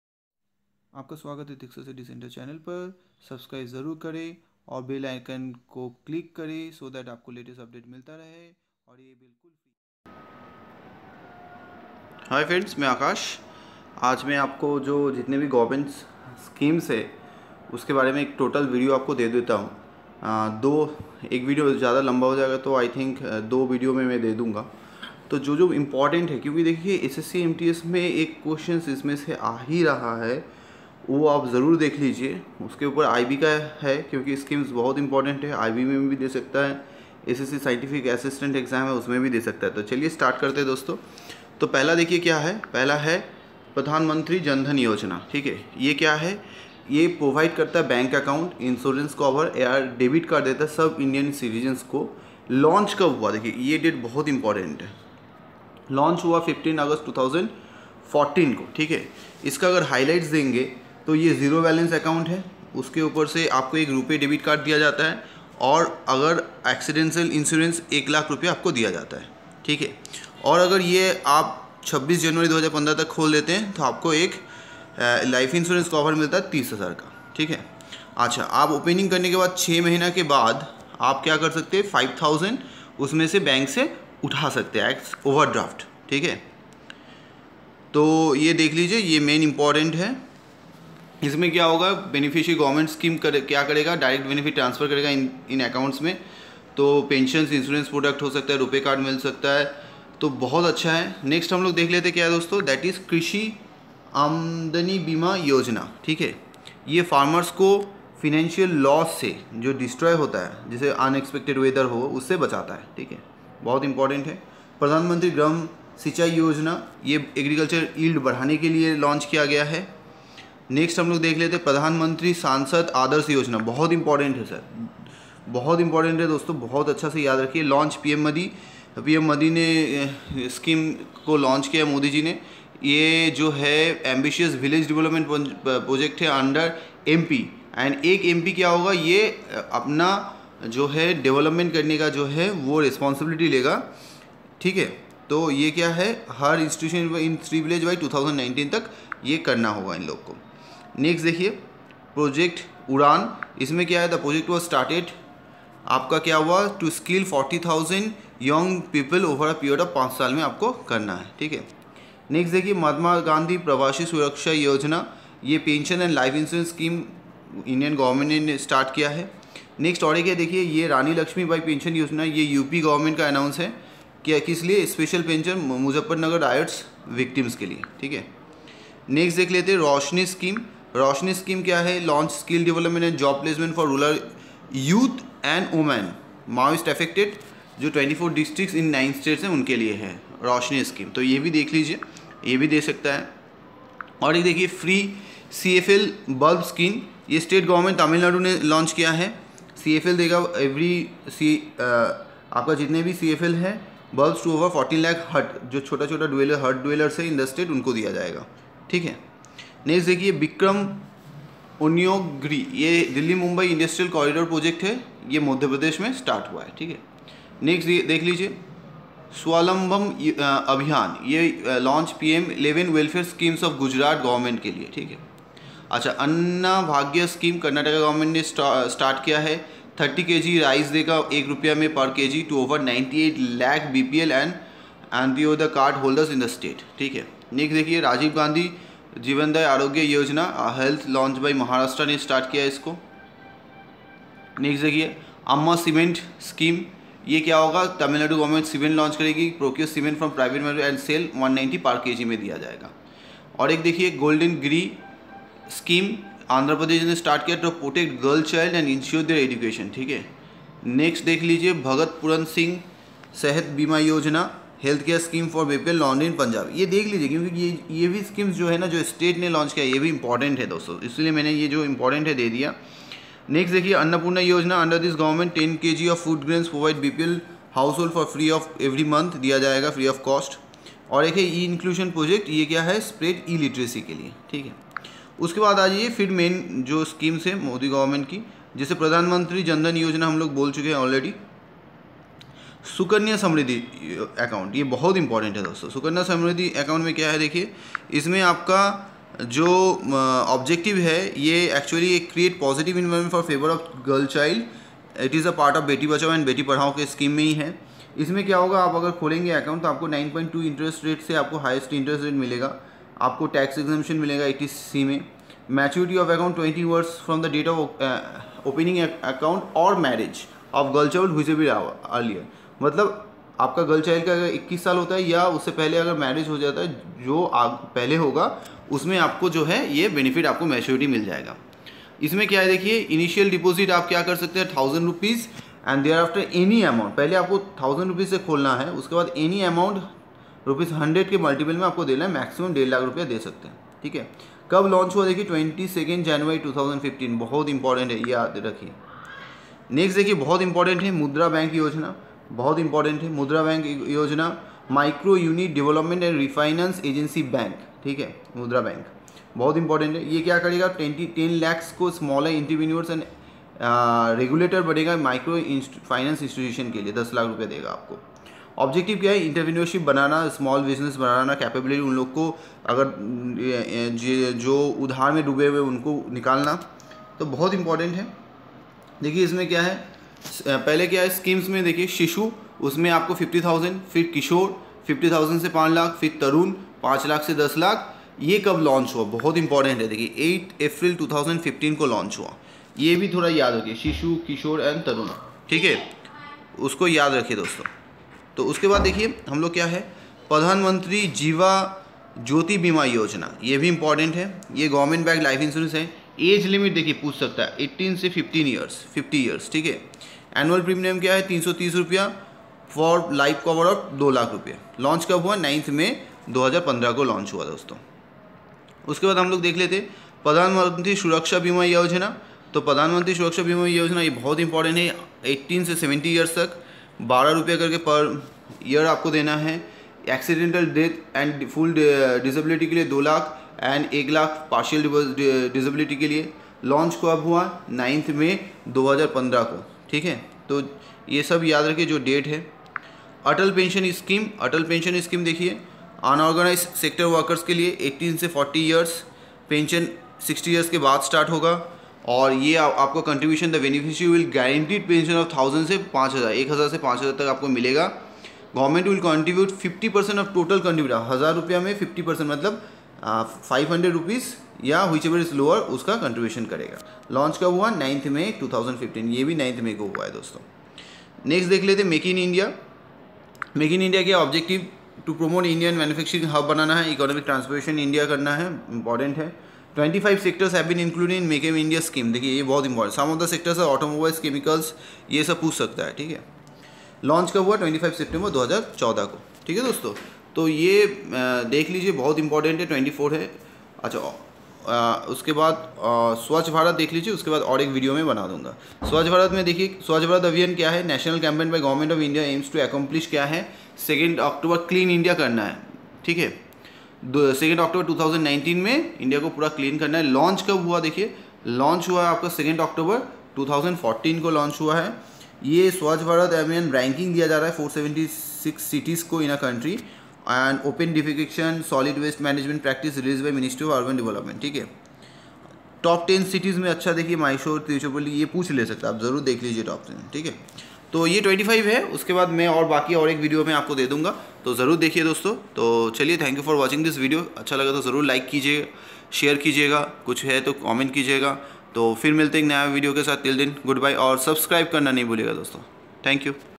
Government Schemes | Part-1 | SSC | RAIL | Others | In Hindi
In this video you will get all the important Government Schemes. It will help you to score good marks. So watch the video till the end.